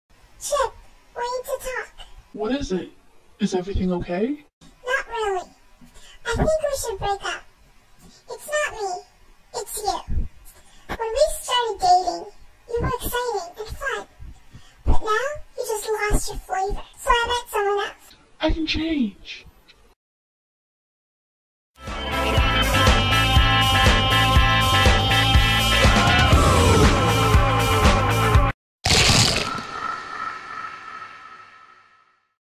Feisty Fiesta Fusion - Doritos are more fun
The fry, who is the girl, breaks up with her boyfriend because he is too boring and he has lost his flavor, unlike Dorito, her new boyfriend. The fry leaves the chip just as he was about to propose. When on a date with Dorito, the fry is having fun until all goes wrong when he's eaten. Dorito was just so irresistible.

Don't be a chip, be a Dorito.
Doritos have more fun!